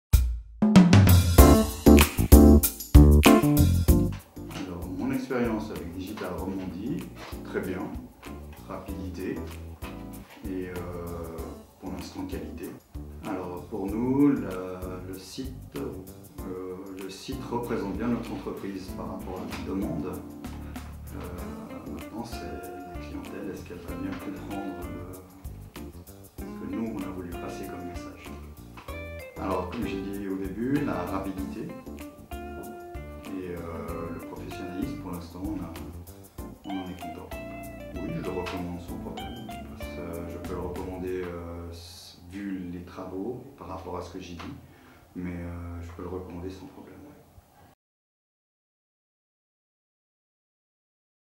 Digital Romandie - Vidéo Témoignage de Locatelli Electricité à Genève
Un grand merci à Jean-Charles Fernandez de Locatelli Eletricité à Genève de nous avoir fait confiance pour la création de son nouveau site Internet.